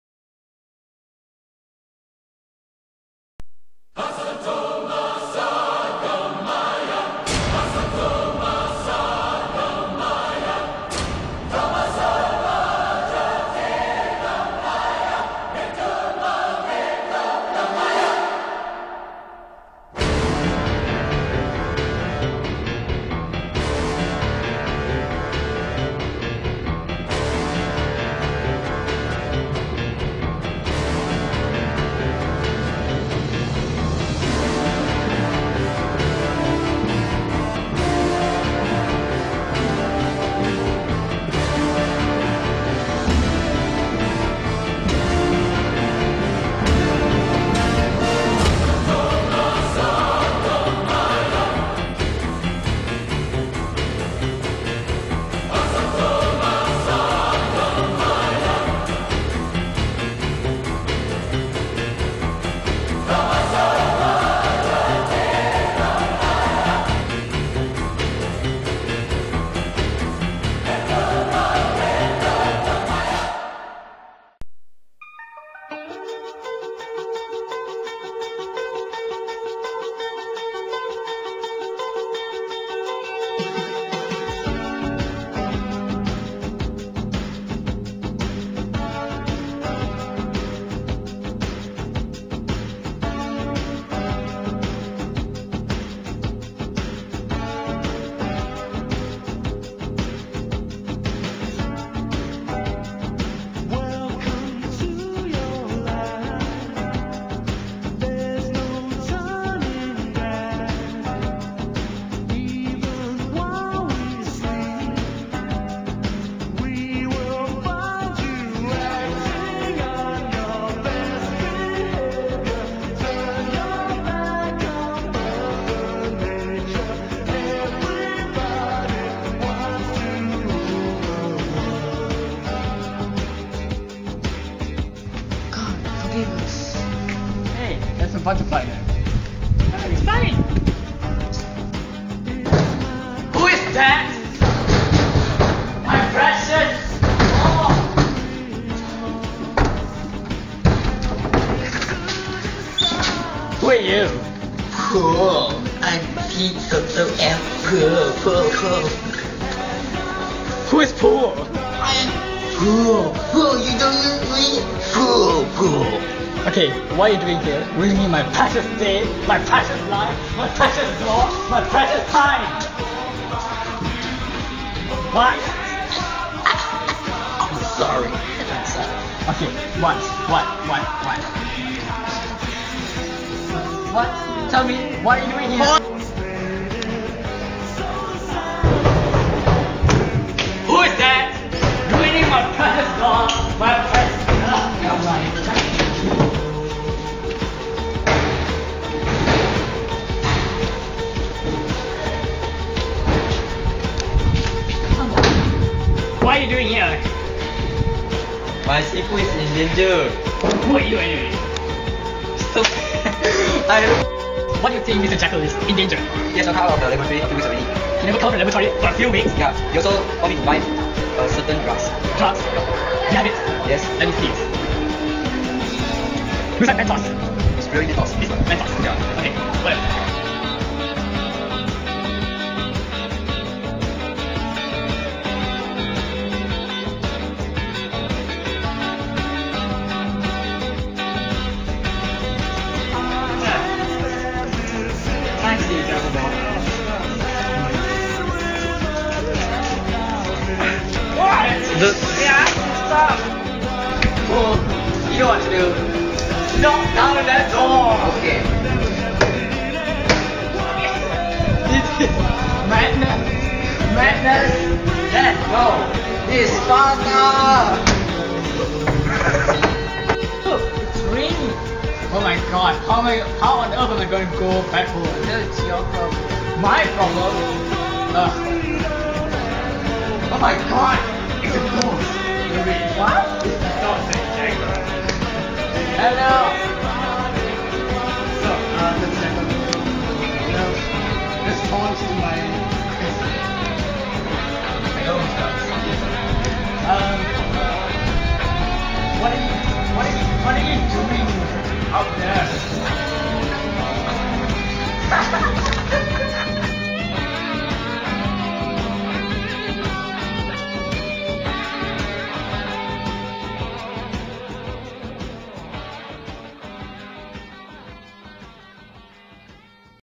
SMKBJ 3 Omega 2009 English Project "Dr.Jekyll and Mr.Hyde" PART 3
This video is a school project based on the novel "Dr.Jekyll and Mr.Hyde". This is the third and final part: Credits and Bloopers...

Basically, this video shows the people involved in filming and making this project. It also shows bloopers!

The location where this video was captured is Sekolah Menengah Kebangsaan Bukit Jelutong. My teammates are all from Form 3 Omega, and we called ourselves "Force 138", deprived from the name "Force 136", a military unit that saw service in Tanah Melayu during WWII...

The video was taken using my friend's MP4 handphone camera, so I apologise for bad video or audio quality. I used Windows Movie Maker to edit it. I tried to make the movie look as proffesional as possible, despite the quality.

I included elements of several movies into this video. I really hope you all enjoy it...